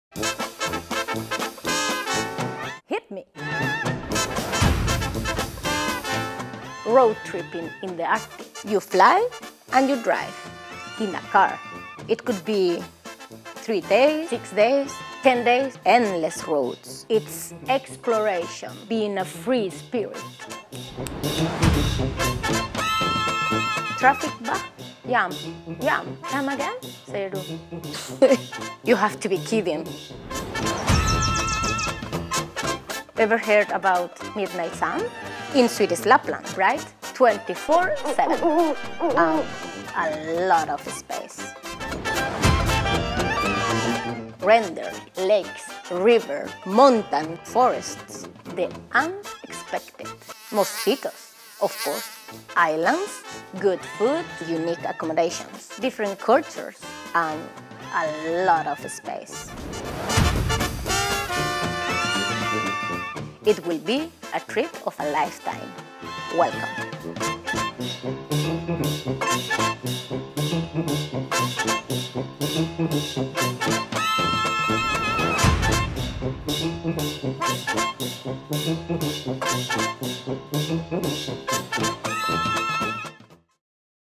Driving in Swedish Lapland
Traffic jams? Dark? What to expect driving in Swedish Lapland? Saleta Beiro answers some of the most frequently asked questions when it comes to road tripping in the Arctic – in her own way.